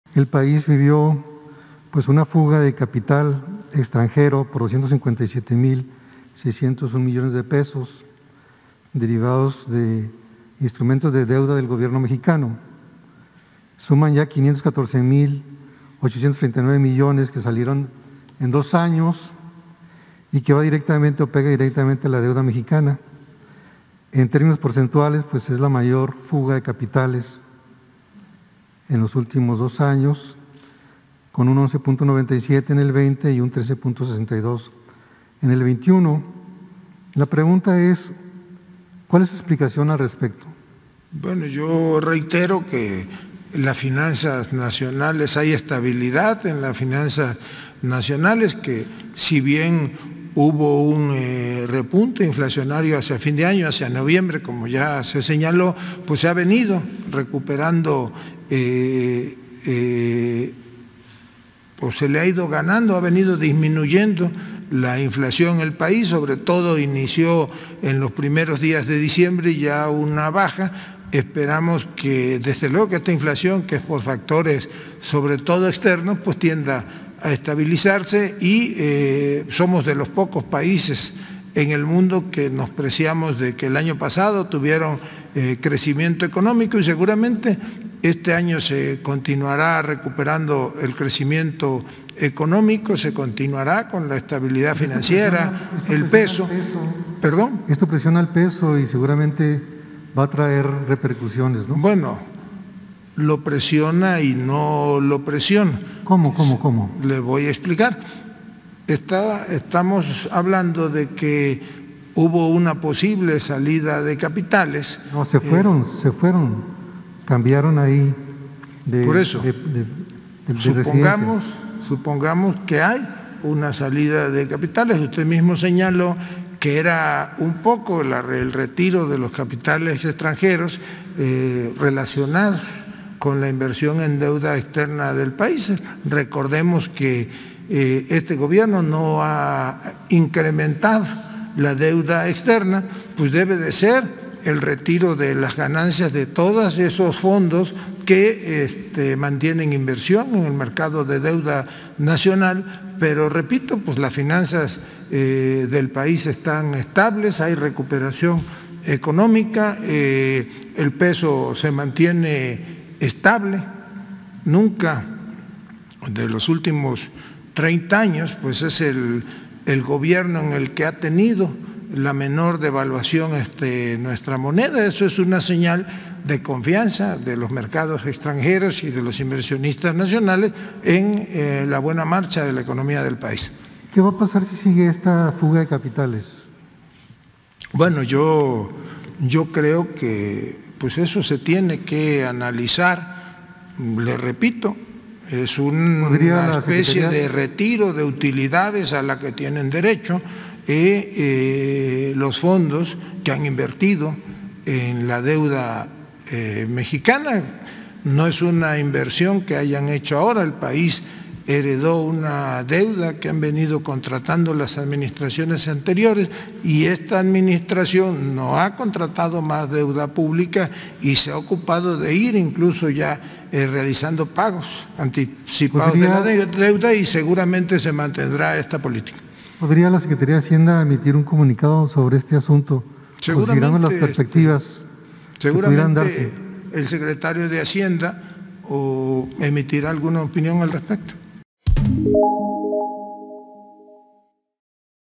“Fuga de capitales”, sin afectar finanzas públicas: Segob
El secretario de Gobernación descartó que la supuesta “fuga de capitales”, por 257 mil millones de pesos en 2021, afecte a la economía.
Y aquí explica los motivos
¿Cómo les quedó el ojo?
"México fue uno de los pocos países que creció en 2021, dato que se estima sea del 5.7 por ciento, por lo que este año continuará la estabilidad financiera".

¿Ya nos sigues en Telegram?